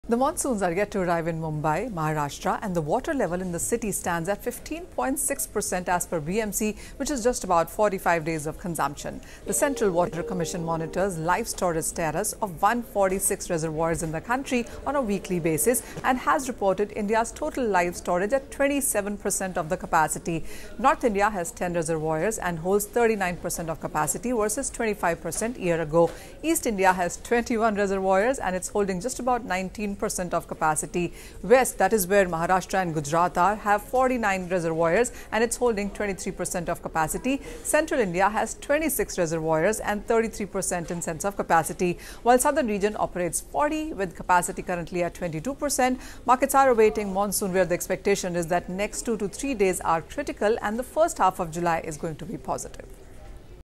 Monsoon Watch | Water Levels Dip In Reservoirs Across Country | Digital | CNBC TV18
While Mumbai patiently awaits the monsoon, data collated by the Central Water Commission shows that live storage capacity in reservoirs across the country is well below the 50% mark. Manisha Gupta is here with more details

n18oc_business